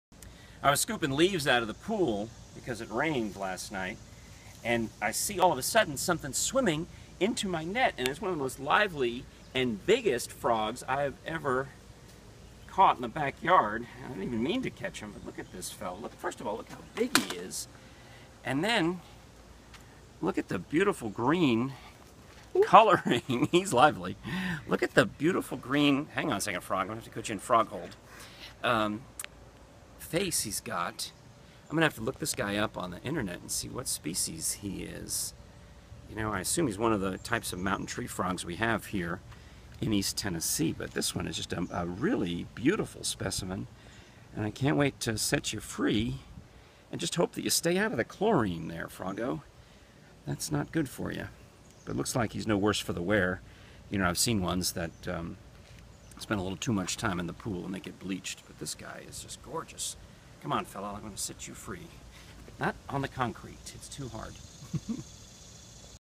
Frank Murphy found a lively frog in the swimming pool.
Frank Murphy found a lively frog in the swimming pool on Father's Day 2015.